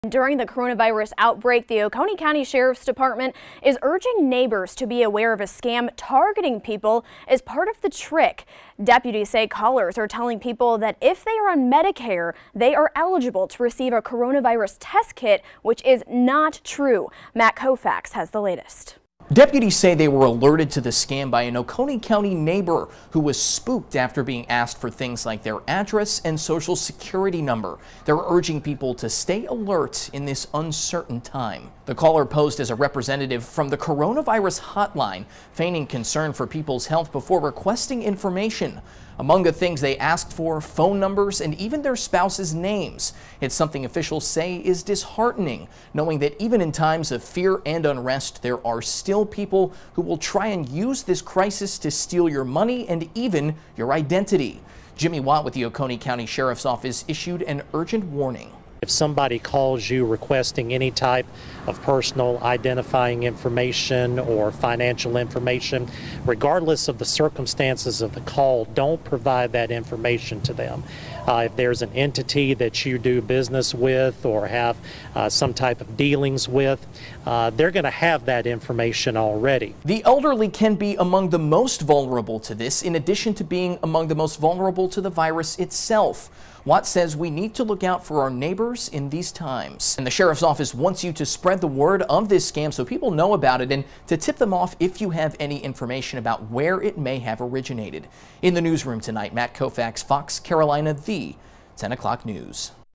Oconee Co. Sheriff's Office warns of coronavirus scams
There are many things to be aware of amid the global pandemic, and the Oconee County Sheriff's Office is now warning residents to be on the lookout for potential scams involving healthcare for COVID-19. (March 17, 2020)